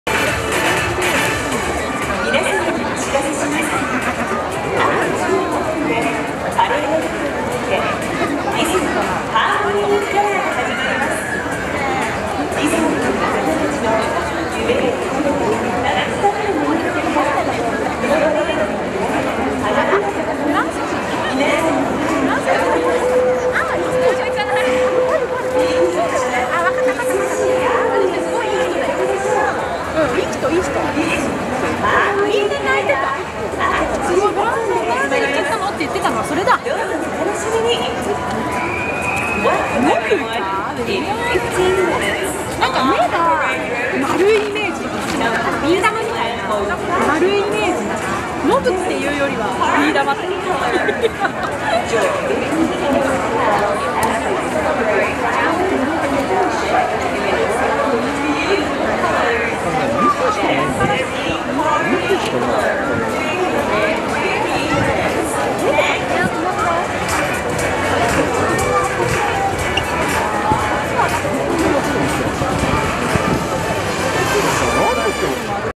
Tokyo Disneyland: 15 Minutes Until Disney Harmony in Color Parade
Saturday, May 20, 2023 - The 15 minute announcement for the Disney Harmony in Color Parade in Westernland at Tokyo Disneyland

For all posts, pictures, videos, thoughts and observations from my May 2023 Tokyo Disney Resort Visit here is the summary